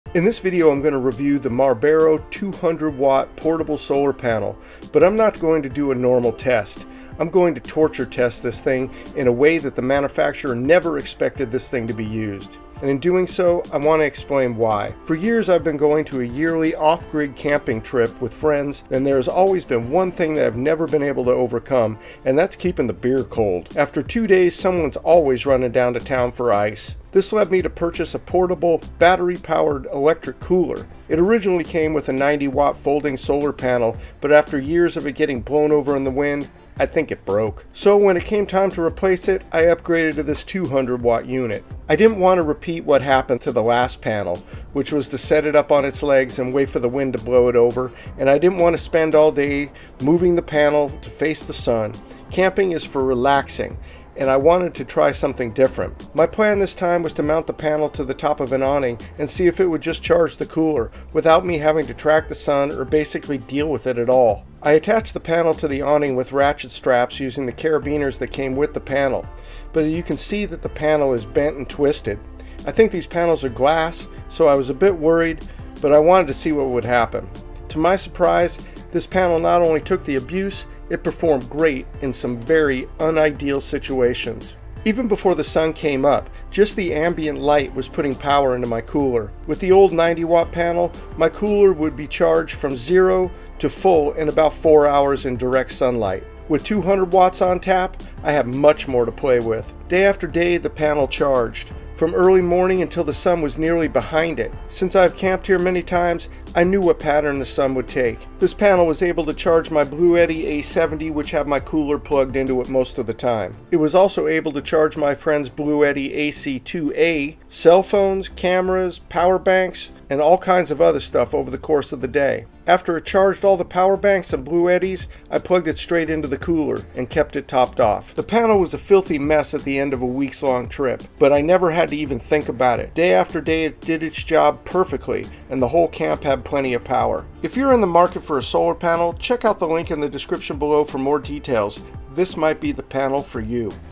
Marbero 200 Watt Solar Panel Torture Test & Review 4k 2
In this video I’m going to review the MARBERO 200W Portable Solar Panel. But I’m not going to do a normal test. I’m going to torture test this thing in a way that the manufacturer never expected this thing to be used.

And in doing so, I want to explain why. For years I have been doing a yearly, off grid camping trip with friends and there has always been one thing I could never overcome and that was keeping the beer cold. After about two days someone is always running down to town for ice.

This led me to purchasing a portable battery powered electric cooler. It originally came with a 90-watt folding solar panel, but after years of it getting blown over in the wind, I think it  broke.

So, when it was time to replace it, I upgraded to this 200 watt Marbero.
I didn’t want to repeat what happened to the last panel, which was to set it up on its legs and wait for the wind to blow over and I didn’t want to spend all day moving the panel to face the sun. Camping is for relaxing, so I wanted to try something different.

My plan this time was to mount the panel to the top of an awning and see if it would just charge the cooler, without me having to track the sun or basically deal with at all.

I attached the panel to the awning with ratchet straps using 4 carabiners that came with the panel, but you can see that the panel is bent and twisted. I think these panels are glass, so I was a bit worried, but I wanted to see what would happen.

To my surprise, this panel not only took the abuse, it performed great in some very unideal situations.

Even before the sun came up, just the ambient light was putting power into my cooler. With the old 90-watt panel my cooler could be charged from zero to full in about 4 hours in direct sunlight. With 200 watts on tap, I had much more to play with.

Day after day, the panel charged. From early morning until the sun was nearly behind it. Since I have camped here many times, I knew what the pattern of the sun would be. This panel was able to charge my Bluetti AC70 which had my cooler plugged into it, which in turn charged my cooler. It also was able to charge my friends Bluetti AC2A, cell phones, cameras and power banks over the course of one day. After it charged all the power banks and Bluetti’s I plugged it straight into the cooler and kept that topped off.

The panel was a filthy mess at the end of the weeks long trip. But I never had to even think about it. Day after day it did its job perfectly and the whole camp had plenty of power.